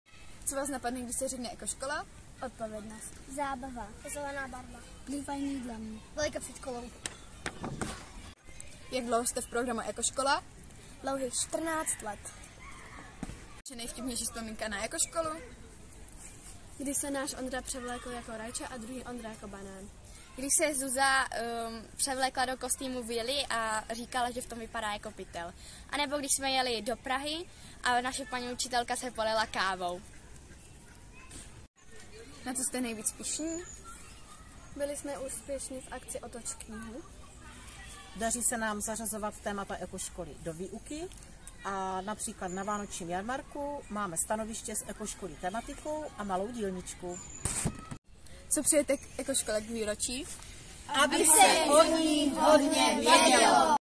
Ekoškolí video výzva od ZŠ Litovel
Ekoškola ve světě slaví již 30 let a na tuto počest vznikla Ekoškolí video výzva, kde školy zapojené do programu odpovídají na naše otázky.
Děkujeme ZŠ Litovel za skvělý filmařský počin. :)